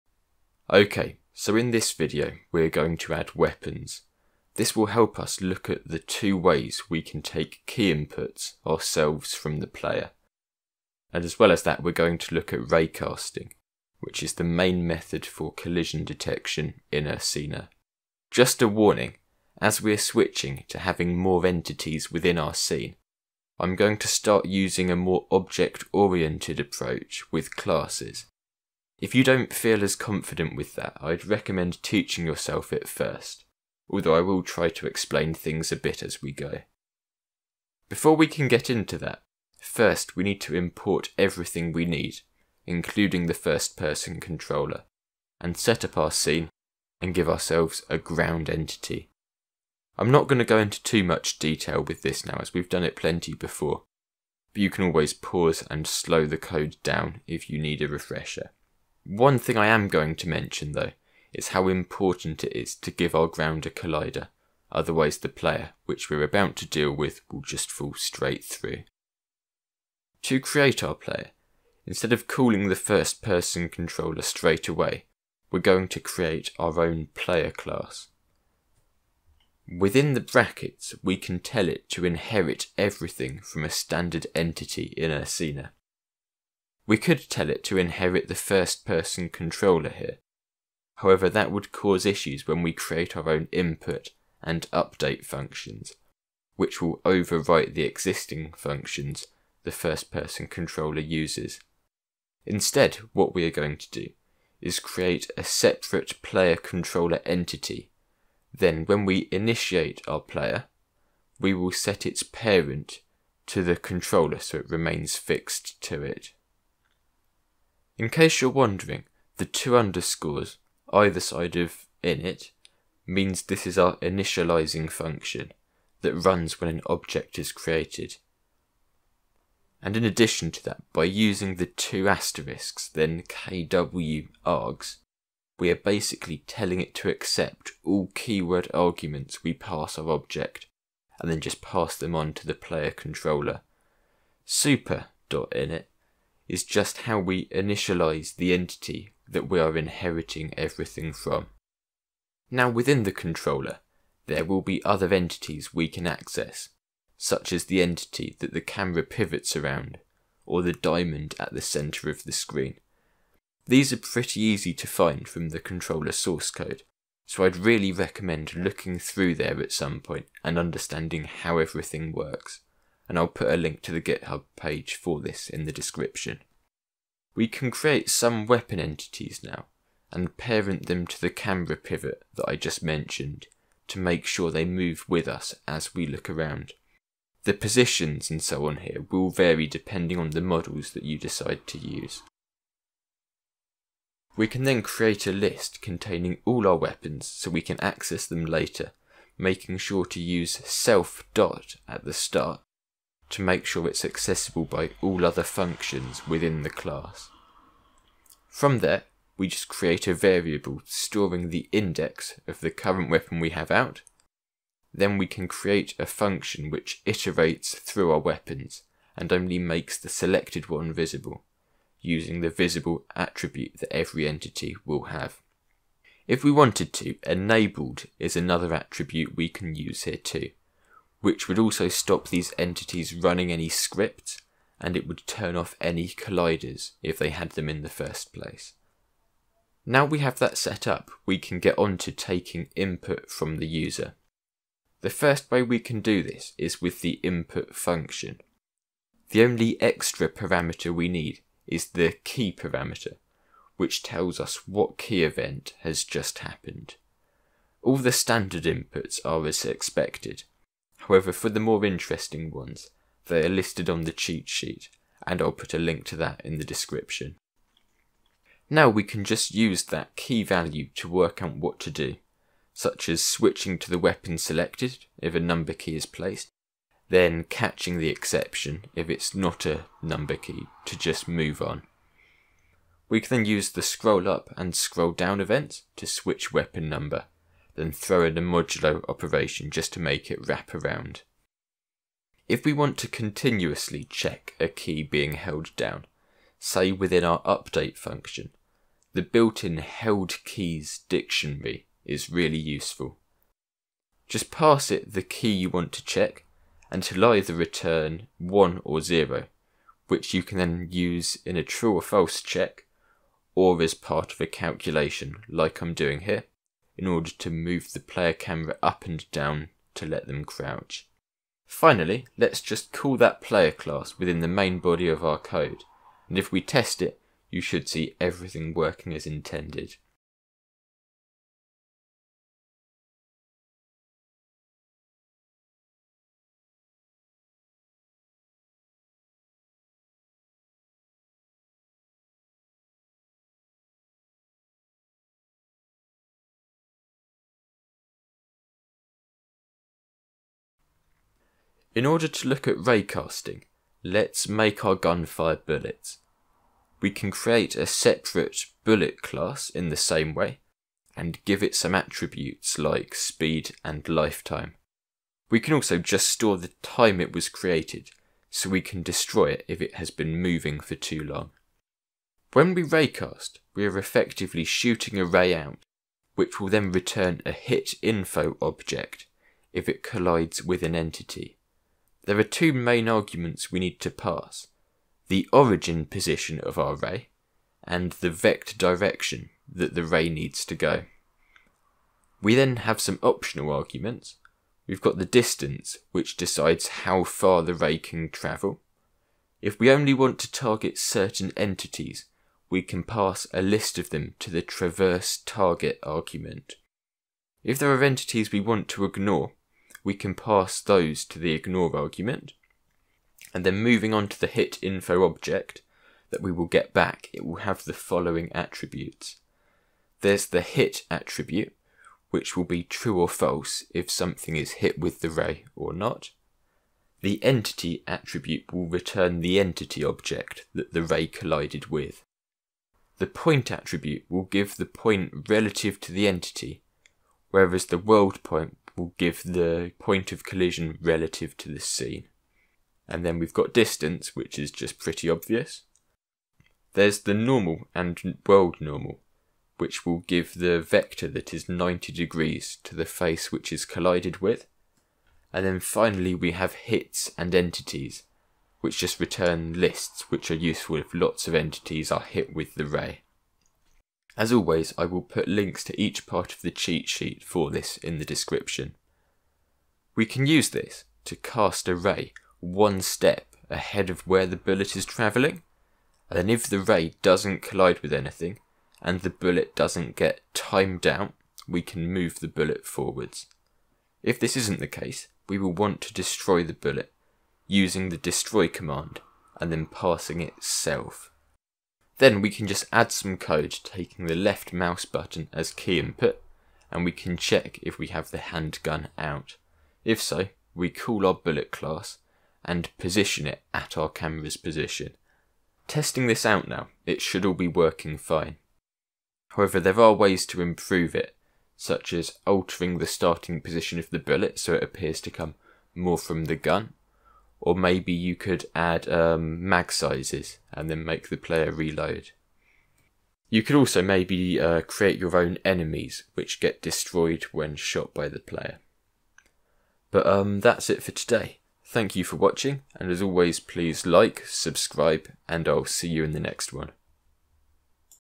3D in Python! Getting Started with Ursina | Ep. 4: Weapons, Input and Raycasting
Looking at Key Input and Raycasting today.
There's a typo right at the end but it's pretty easy to spot.